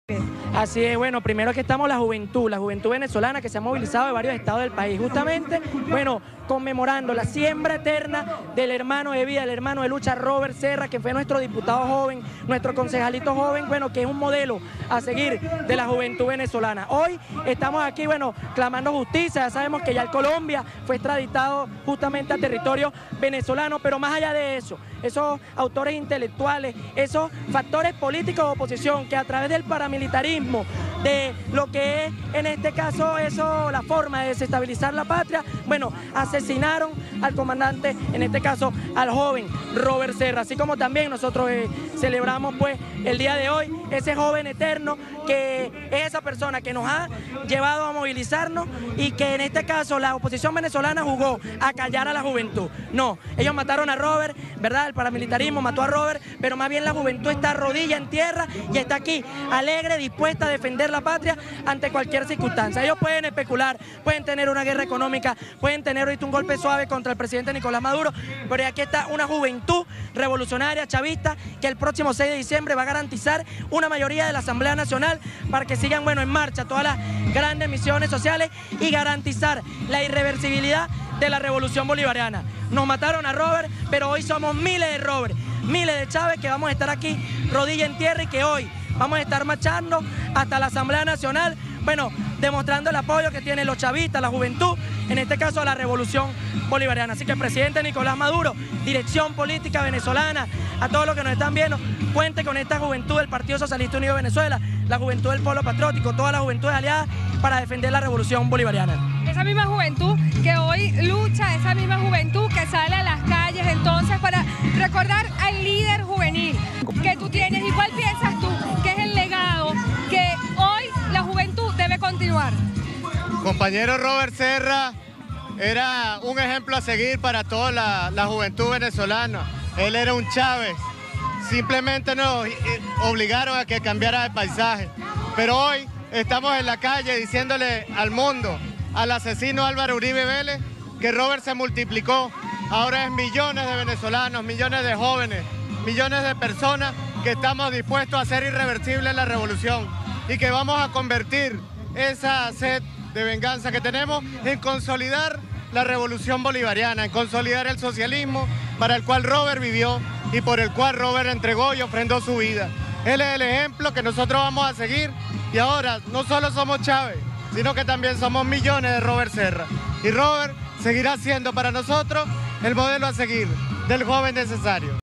Jóvenes patriotas inician en Caracas concentración en honor a Robert Serra
Créditos: VTV